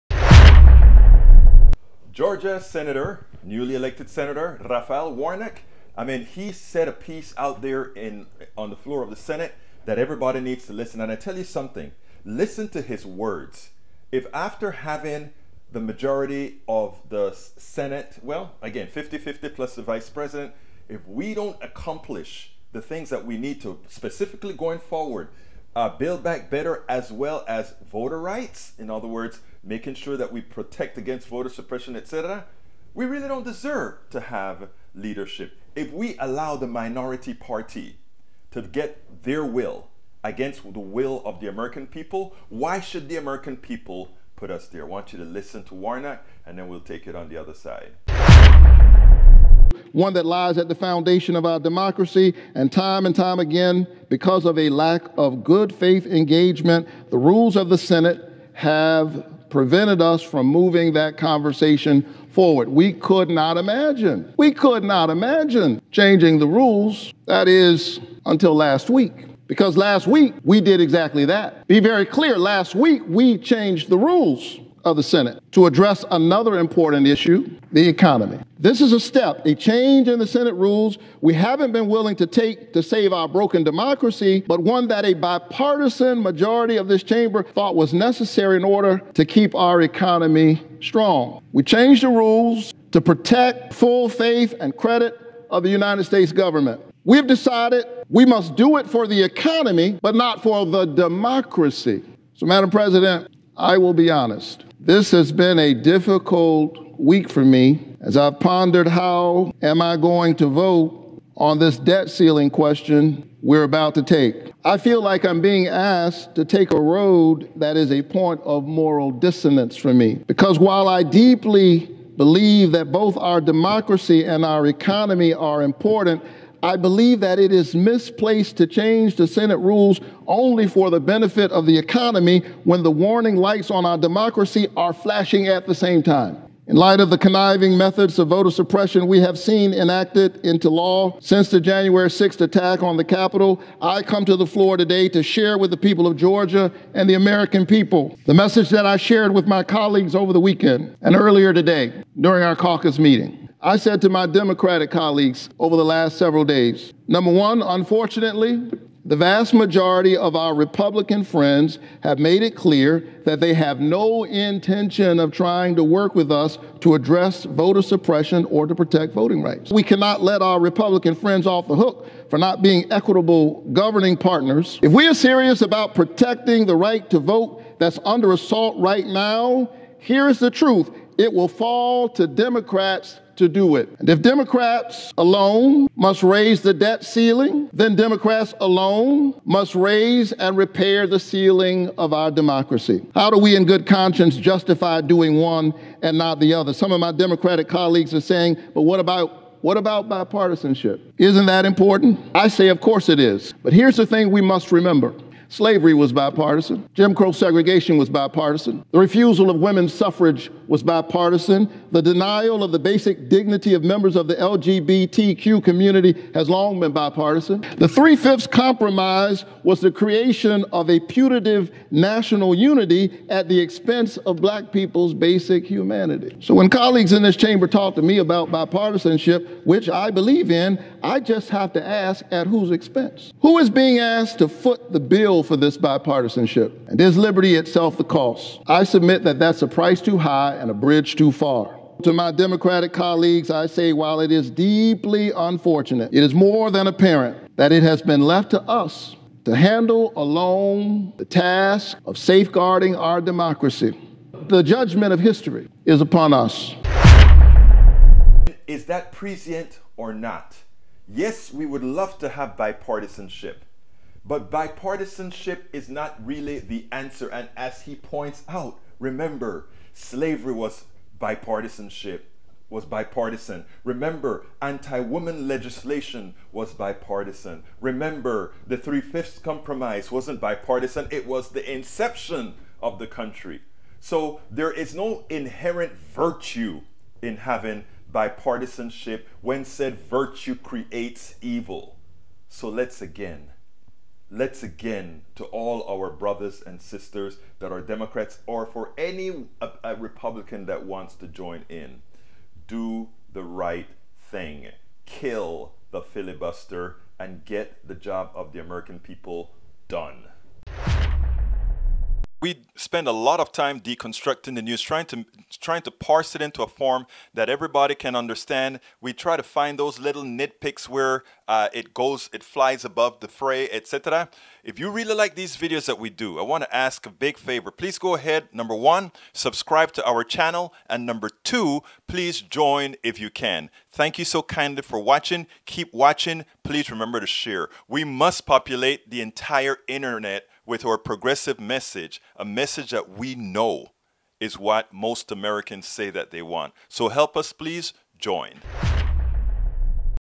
Sen. Raphael Warnock (D-GA) reminds us that slavery & voting rights denial were all bipartisan.
In one of the best floor speeches in some time, Georgia Senator Raphael Warnock implored Democrats to act on voter rights.